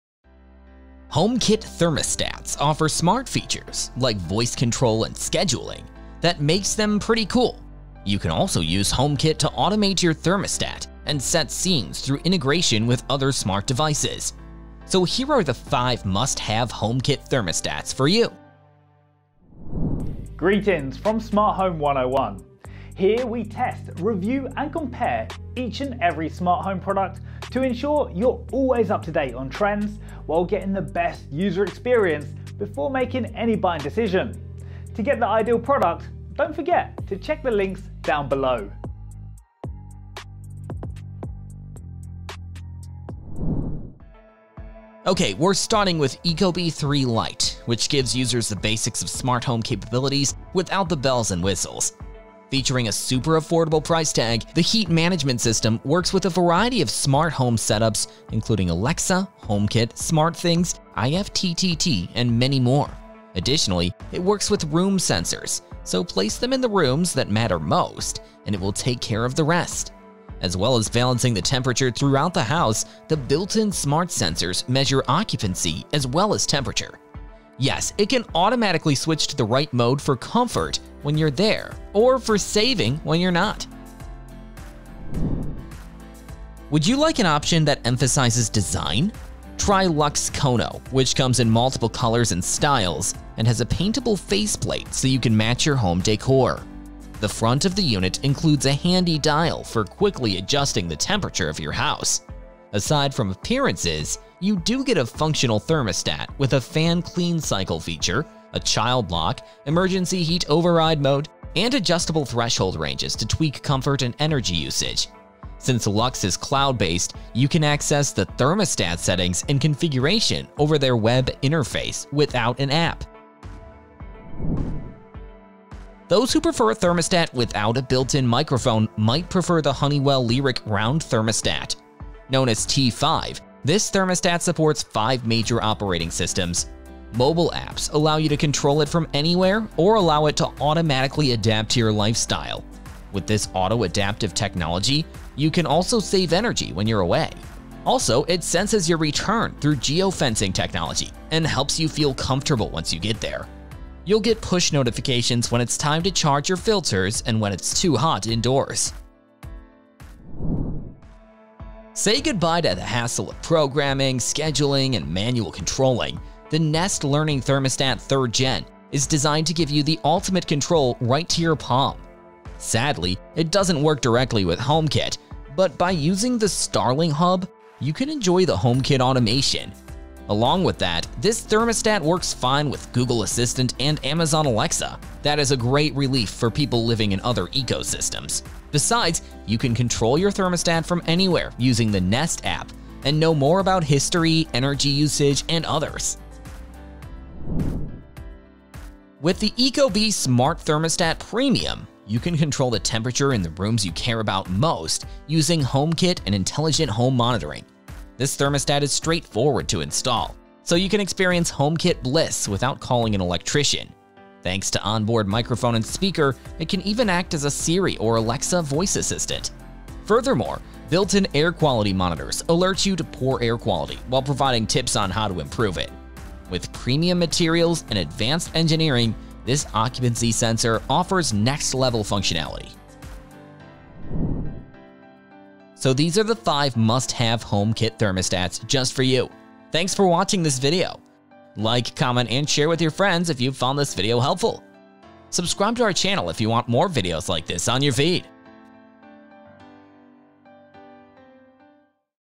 5 Must Have HomeKit Thermostat For Smart Home
HomeKit thermostat adds many modern conveniences, like Siri voice control, and remote controls, to your home’s heating and air system. They also offer a way to set your home’s heating and cooling in many ways. So, here is a list of 5 must-have HomeKit thermostat that you should check now!